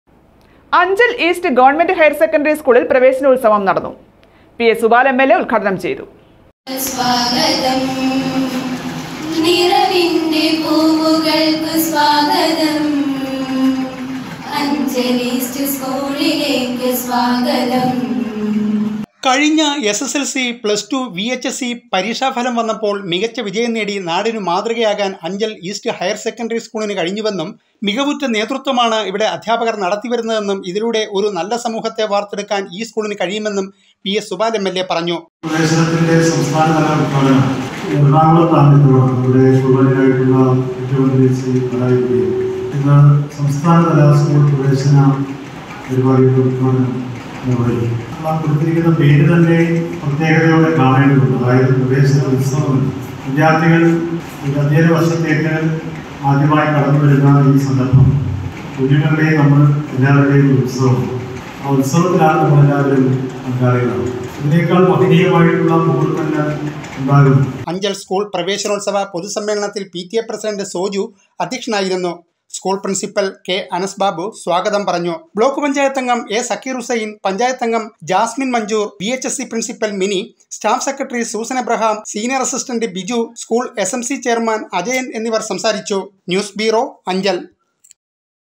സ്‌കൂളില്‍ പ്രവേശന ഉത്സവം
അഞ്ചല്‍ ഈസ്റ്റ് ഗവ.ഹയര്‍ സെക്കന്‍ഡറി സ്‌കൂളില്‍ പ്രവേശന ഉത്സവം നടന്നു. പി .എസ് സുപാല്‍ എം എല്‍ എ ഉദ്ഘാടനം ചെയ്തു.